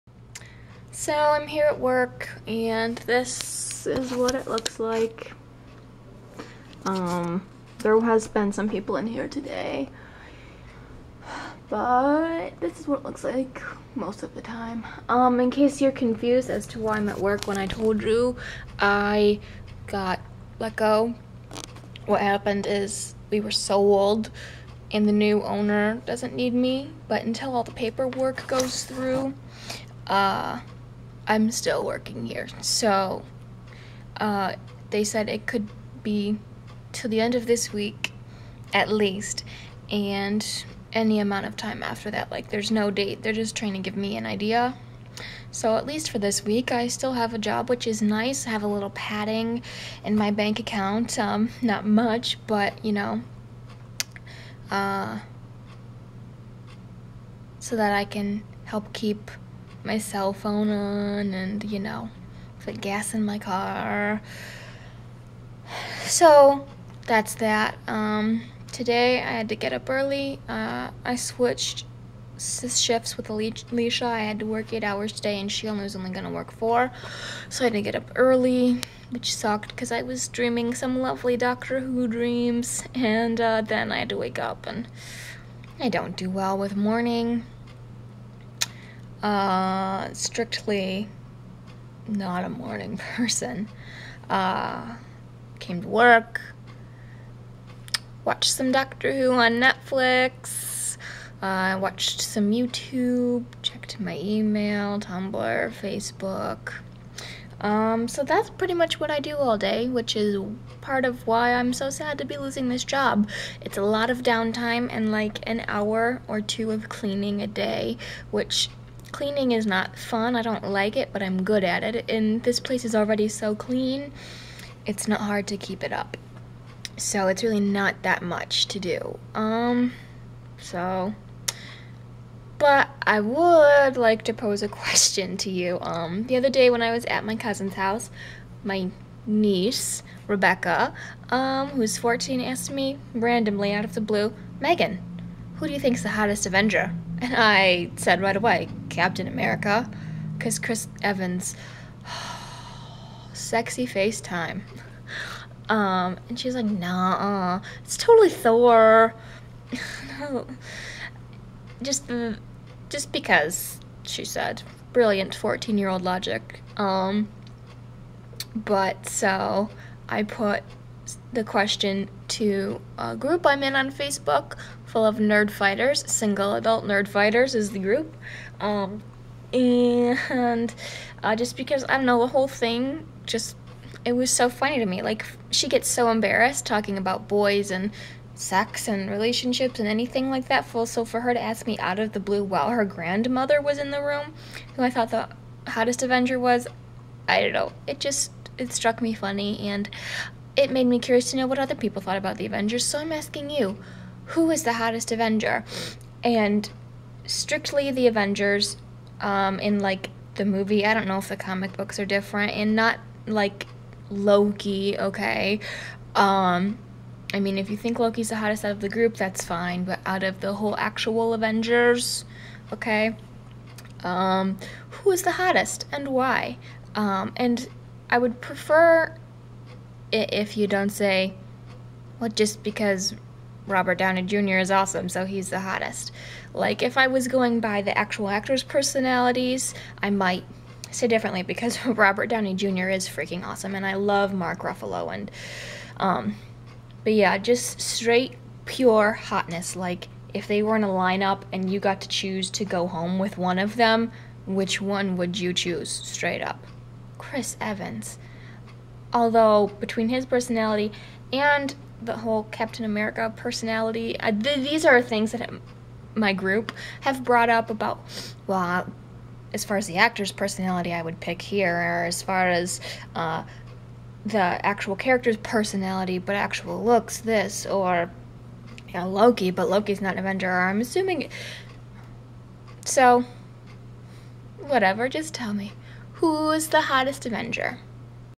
VEDO - Oct. 3: Who's the hottest Avenger?
In which I briefly speak about my day, and then pose a question that has been plaguing us all: Who is the hottest Avenger?!